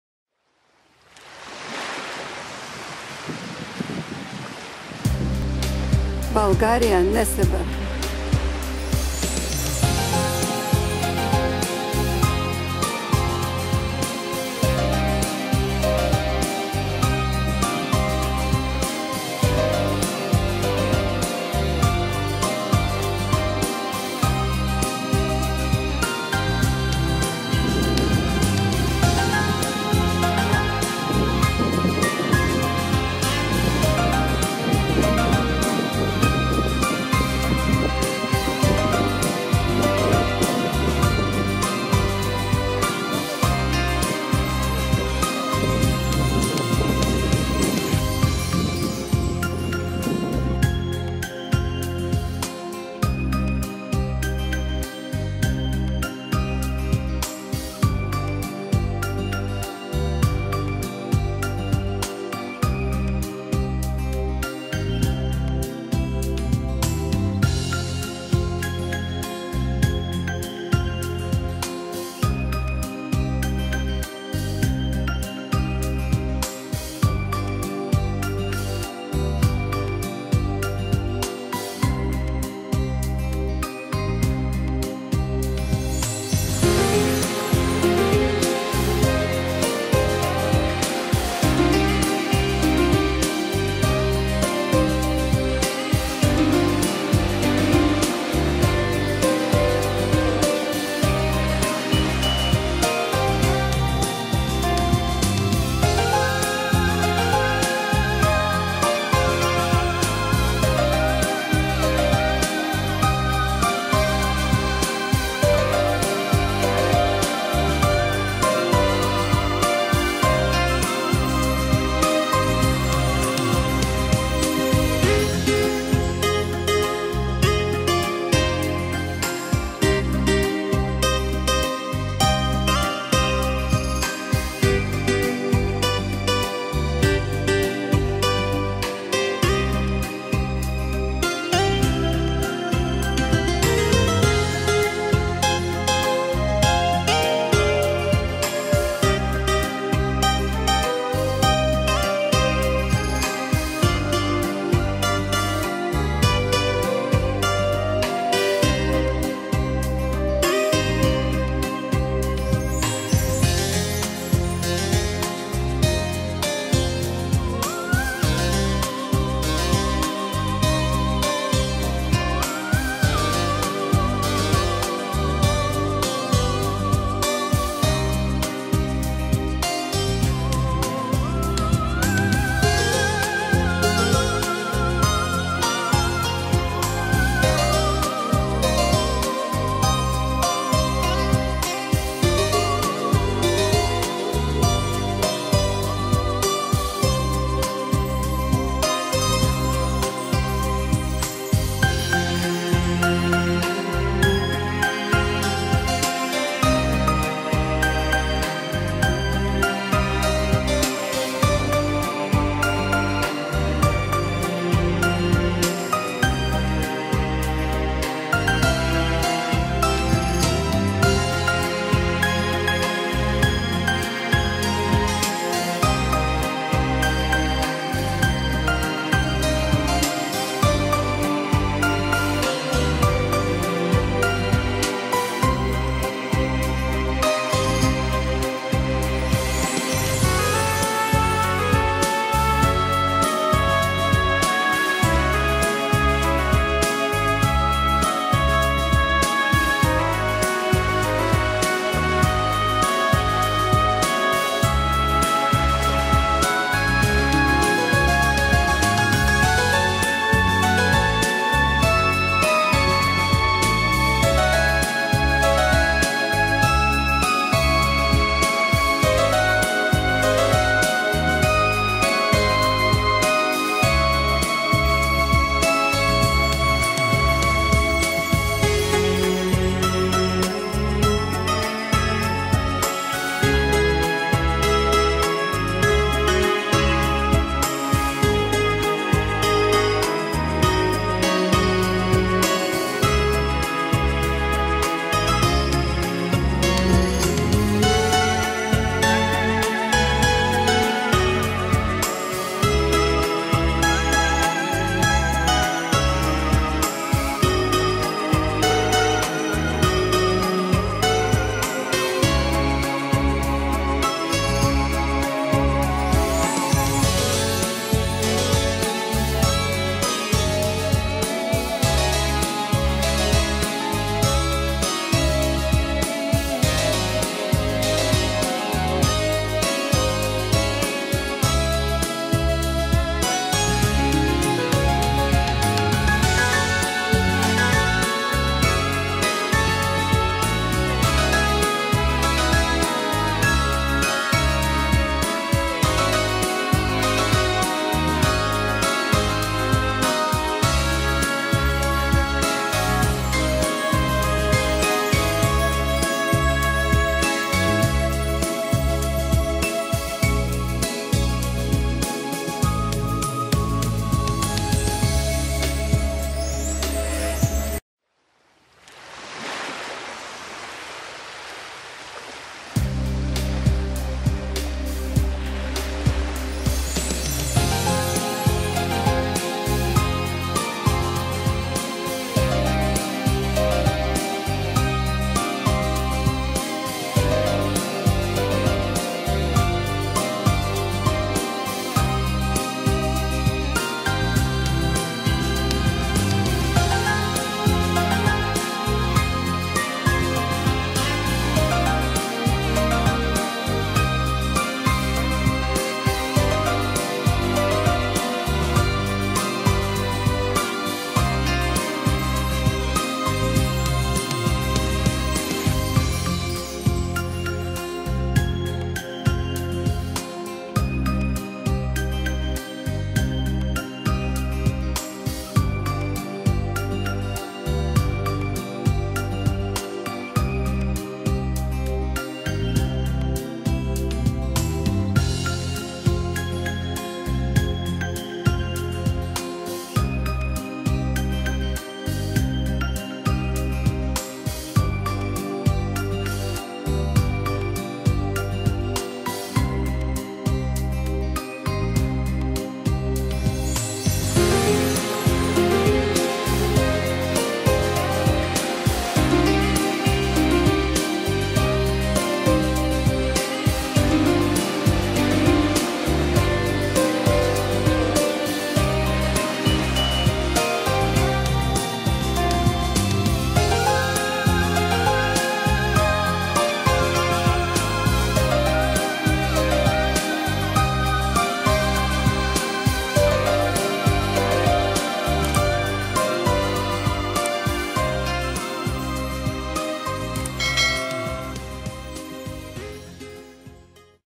БОЛГАРИЯ Несебр_Солнечный_берег MusikMix Незабываемыйотпуск Bulgarien_Nessebar_Sonnenstrand
Старый город Несебр - одна из важнейших
достопримечательностей Болгарии как город-музей с уникальной атмосферой, исторически ценными церквями и прекрасно отреставрированными домами
Несебр, как город-музей и объект всемирного наследия ЮНЕСКО, является неотъемлемой частью туристической программы каждого отпуска в Болгарии.
Mus. S. Chekalina
видео и фотографии: Musik Mix 66
монтаж:  Musik Mix 66